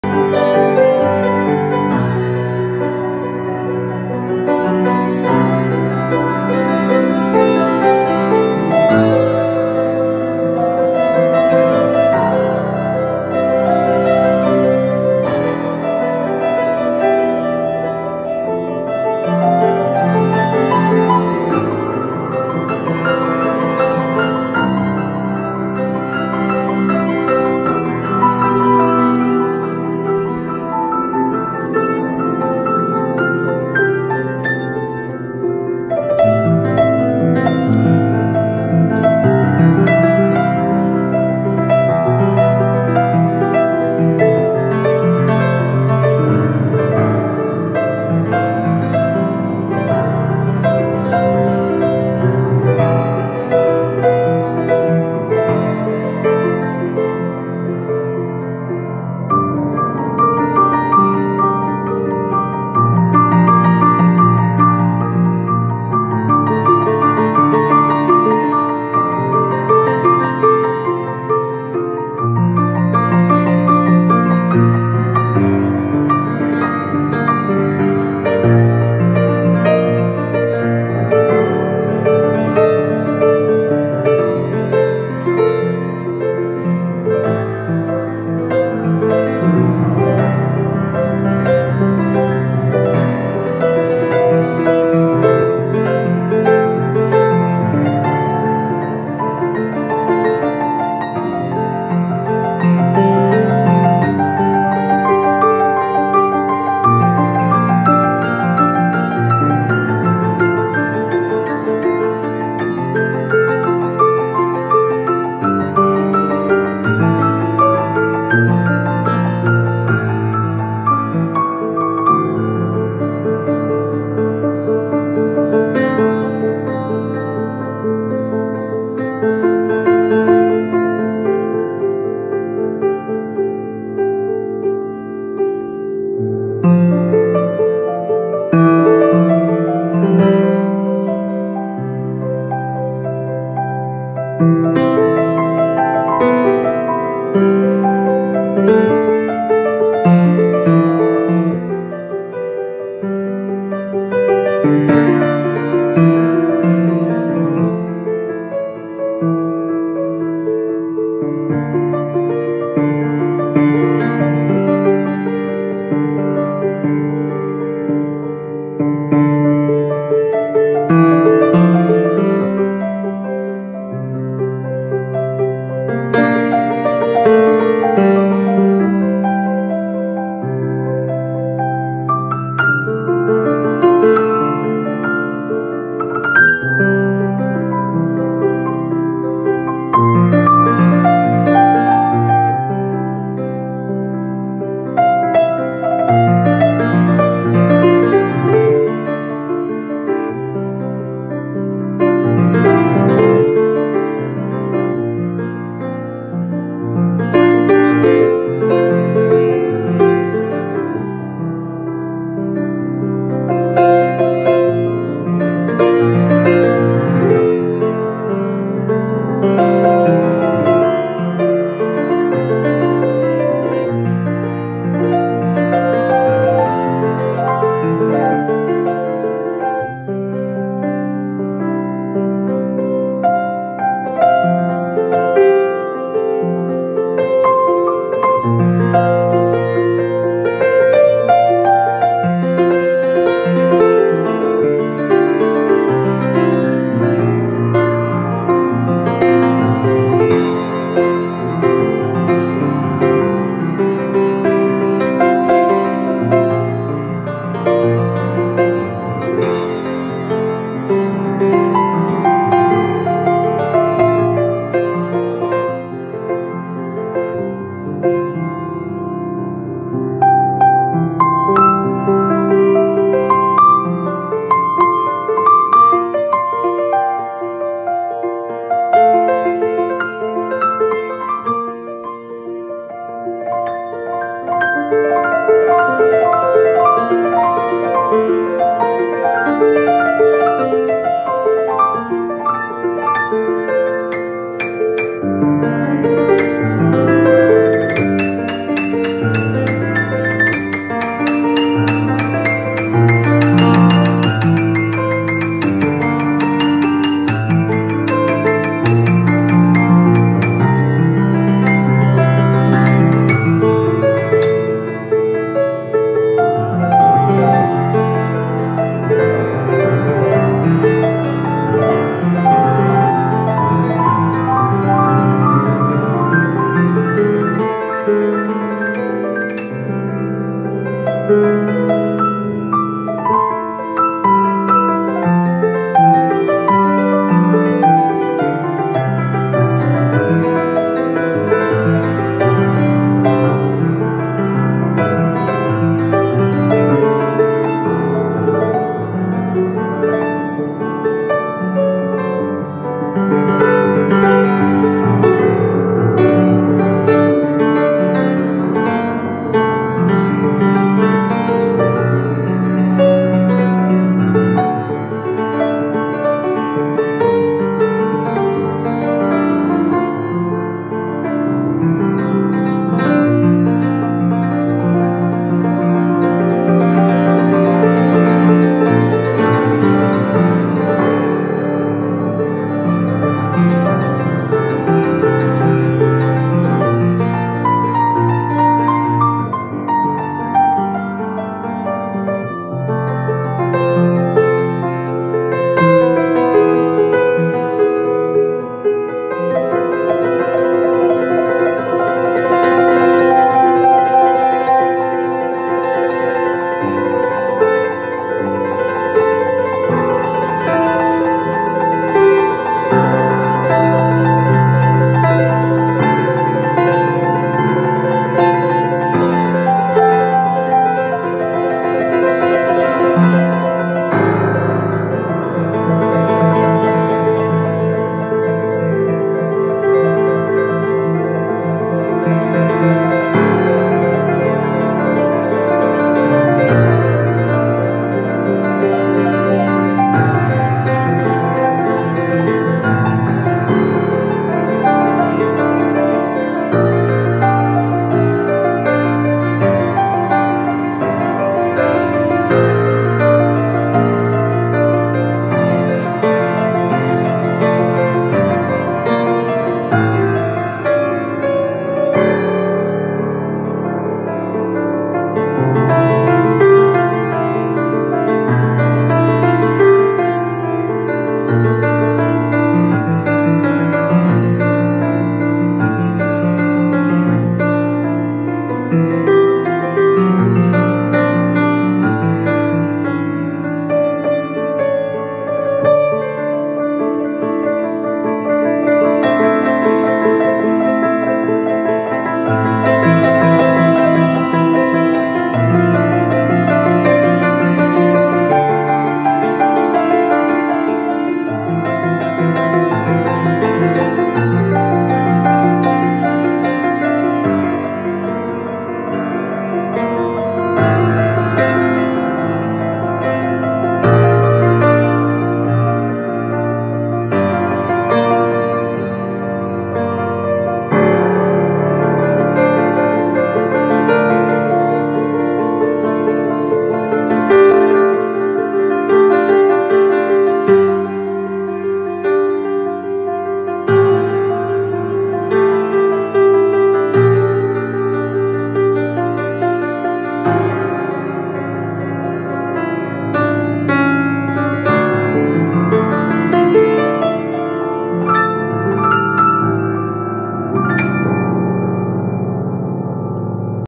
Pearl River baby grand piano - original work by J. Michael Niczyporuk
Original composition by J. Michael Niczyporuk performed on a Pearl River baby grand piano in Hayes Valley, San Francisco.